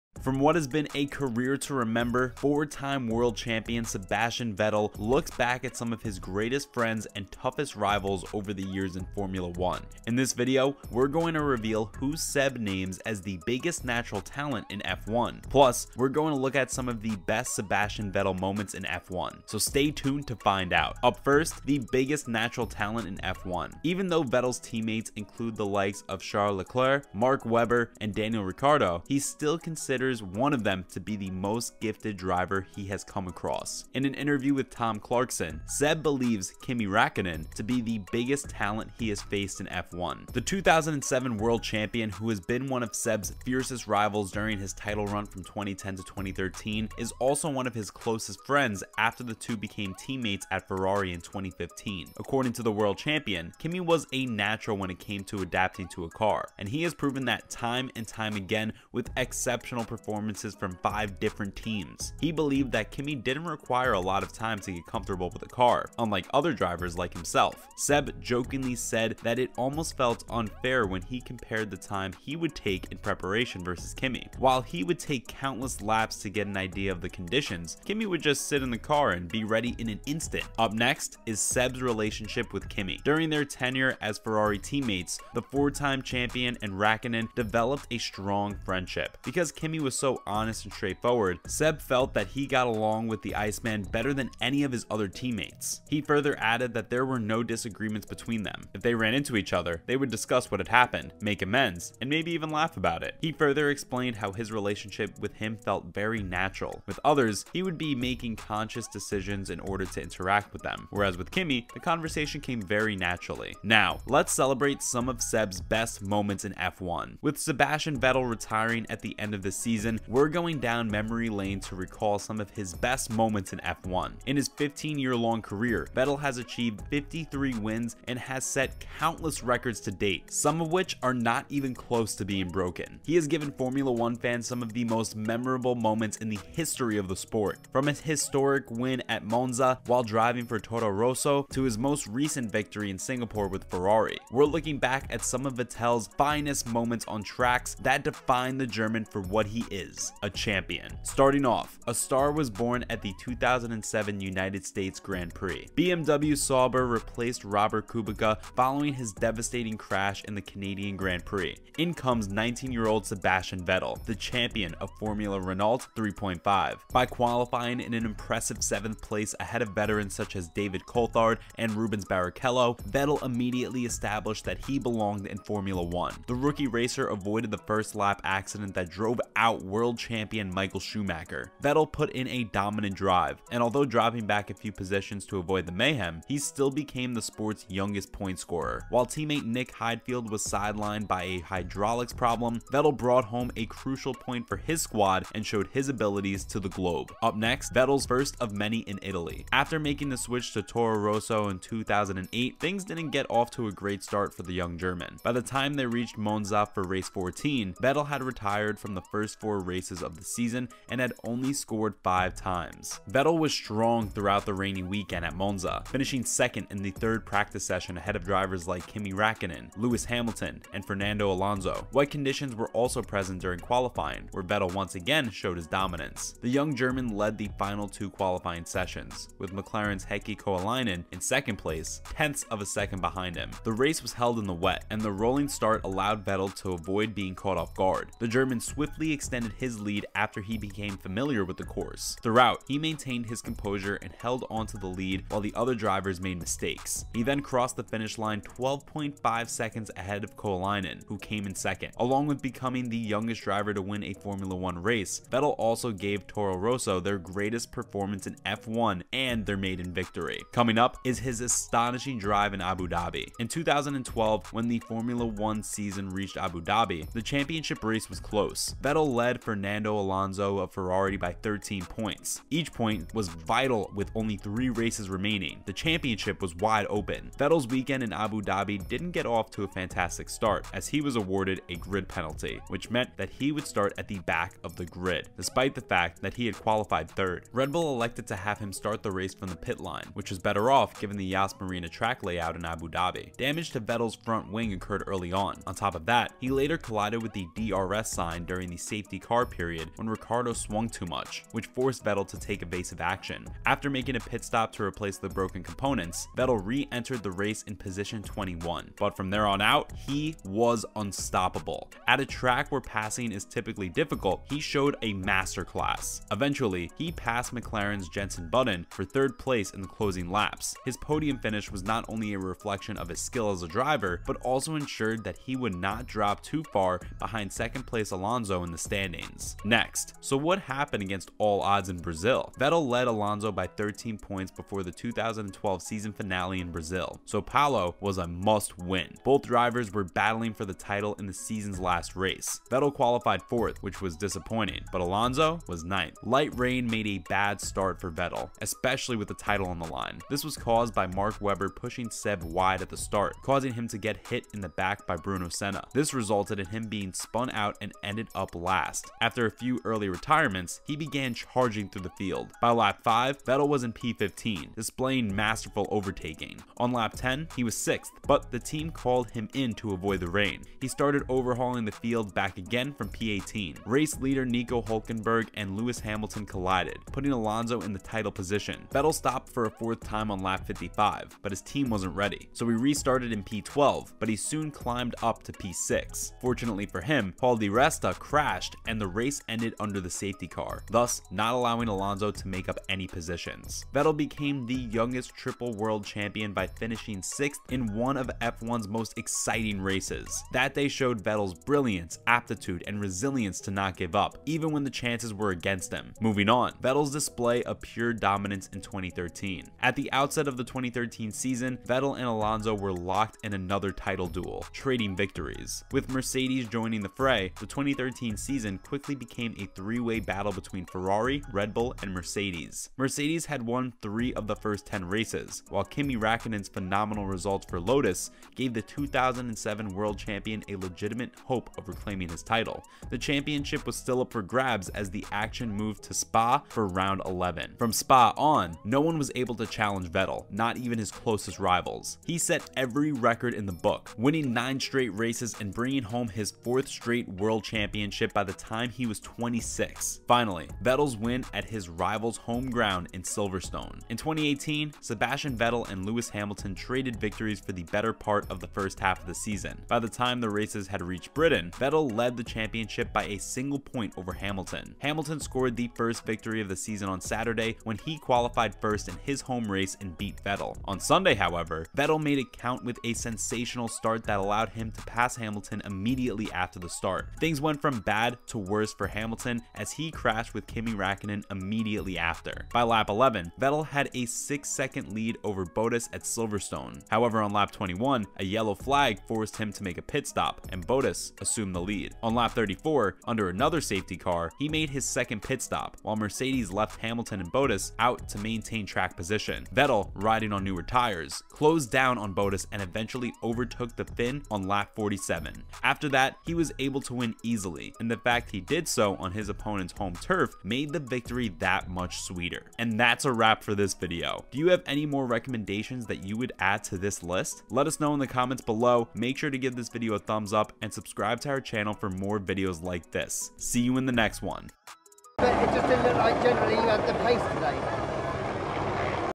Sebastian Vettel REVEALS The BEST Racer He's Seen..
Welcome back to Sports Stopper. Sebastian Vettel is about to hit the brakes on his Formula 1 career and, in the latest episode of Beyond The Grid, the four-time F1 champion opens up about his greatest friends and foes in the sport that has been his life for the past 15 years… He's the man of the moment as he readies for what is set to be his final race in Formula 1, and the German driver discusses every facet of his career in this unmissable interview with Tom Clarkson. Vettel has had to contend with the likes of Charles Leclerc, Mark Webber, and Daniel Ricciardo as teammates, but he singles out one driver as being the best when it comes to all-out speed.

The four-time champion adds that he and Raikkonen enjoyed a solid relationship during their time as teammates at Ferrari from 2015 to 2018. "I think with him, I probably had the best relationship out of all the teammates I had, because he was just so straightforward. There was never an argument. If we crashed into each other we talked about it, fixed what happened, maybe laughed about it…"But there was never a question that anything could sort of shake up or destabilize the, I don’t want to say bond, but the relationship that we had. He’s probably also the one when I came in, I remember, he was so respectful from the day I walked in, looking into my eyes. With other drivers, I felt, 'OK I’m shaking hands, I’m saying hello, but actually the guy’s not present, he’s not here. "With people, I think Kimi’s been exceptional." In related news, Take a look at ten of Vettel’s finest moments on track, which define the German for what he is: a champion. USA 2007: A star is born, Italy 2008: The first of many, Abu Dhabi 2012: A class apart, Brazil 2012: Against the odds, 2013: Nine in a row — Untouchable, Singapore 2013: Dominance of another kind, Bahrain 2018: Managed to perfection, England 2018: Victorious in hostile territory, Germany 2019: A charge through the field, & Turkey 2020: Silver lining.